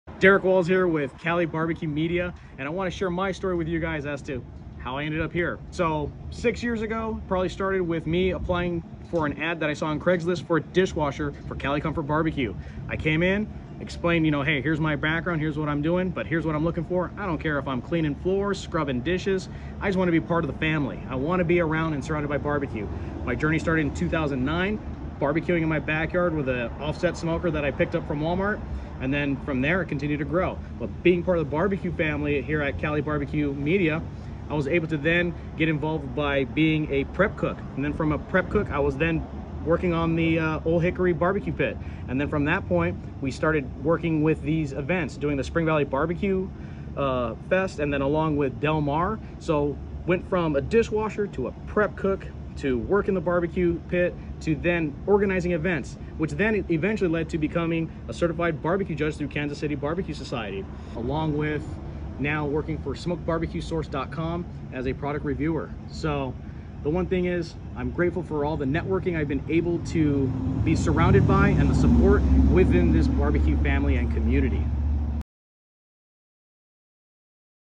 My BBQ Story featuring Derrick Walz | Behind the Smoke at Cali BBQ
Cali BBQ's Derrick Walz (@derrickwalz) shares his barbecue journey — from dishwasher to prep cook to pitmaster to a certified Kansas City Barbeque Society judge.